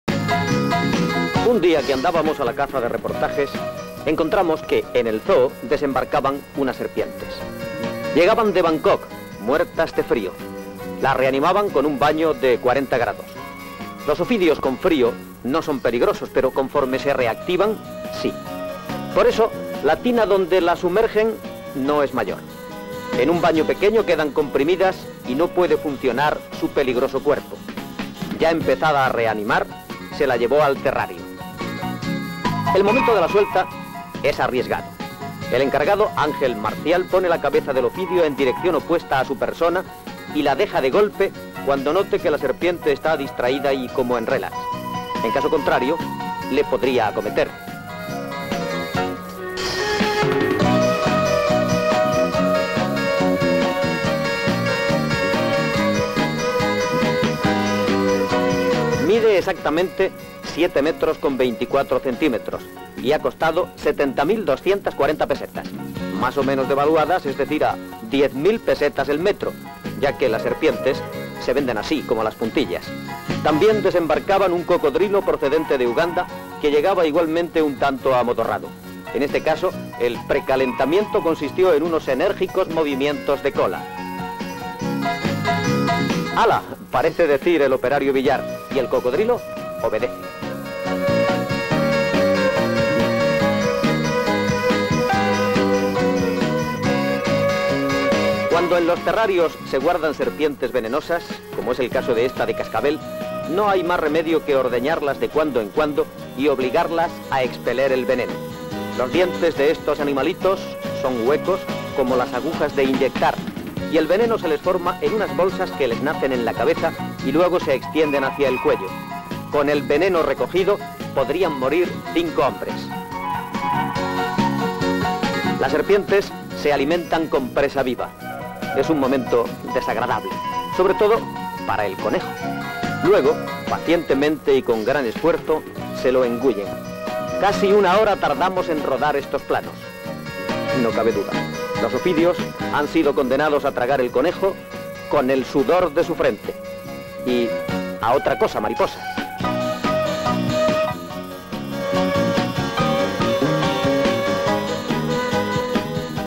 Llegada de nuevas serpientes y reptiles al Zoo de Madrid (1977) - Terrarios, serpiente de cascabel
Llegada de nuevas serpientes y reptiles al Zoo de Madrid (1977)